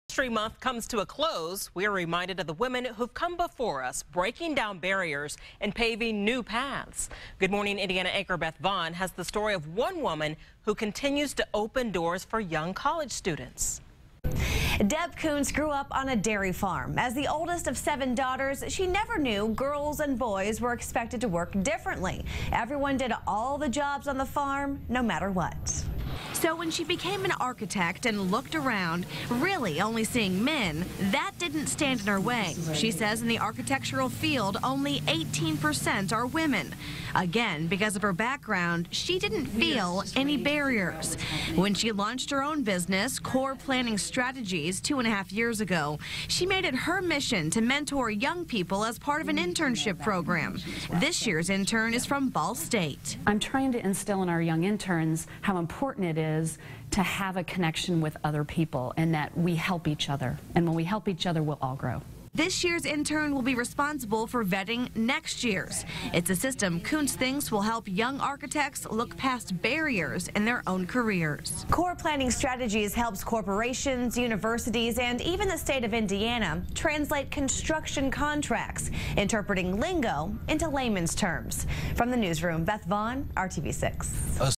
Indy business owner helps students get their start
One female business owner continues to open doors for young college students.